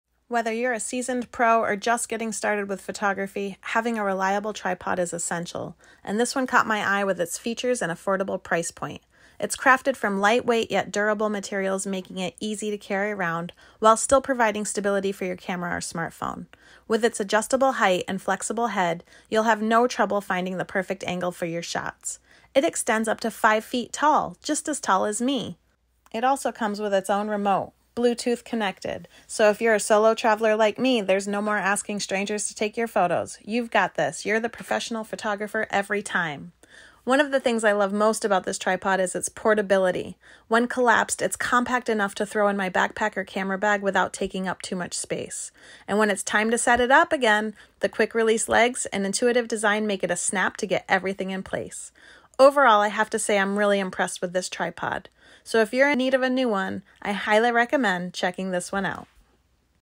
BEST Travel Tripod for Smartphones REVIEW & DEMO
Feel like a professional photographer every time! No need to ask strangers to take your picture anymore! Group pics won't ever leave anyone out again with this bluetooth remote controlled tripod!

I'm moving to Spain at the end of July, so look forward to fun content around that adventure!